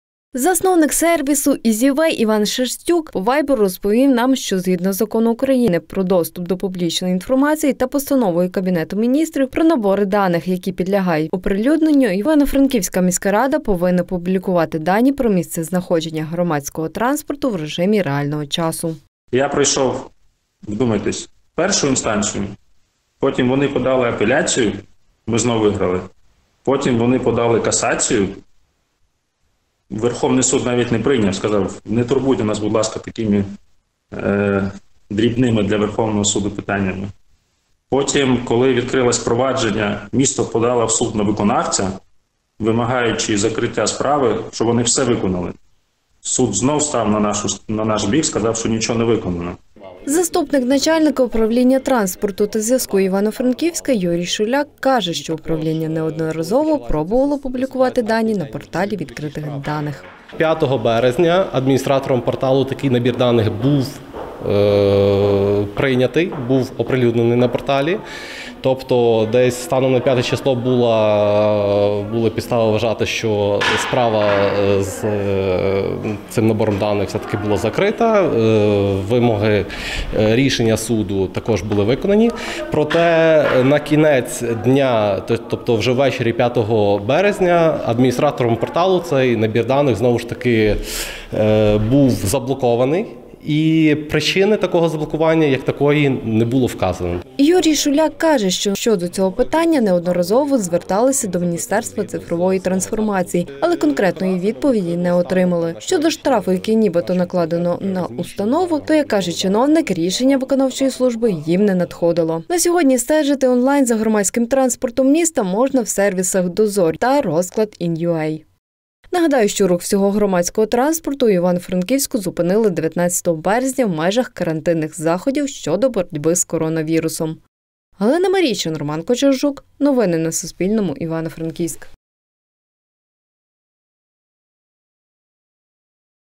Франківська влада заплатить штраф за недоступ до транспортних даних в режимі реального часу
П’ять тисяч 100 гривень штрафу заплатить Івано-Франківська міська рада за те, що не надає доступу до транспортних даних в режимі реального часу. Про це кореспондентам Суспільного повідомив засновник сервісу Easy Way Іван Шерстюк.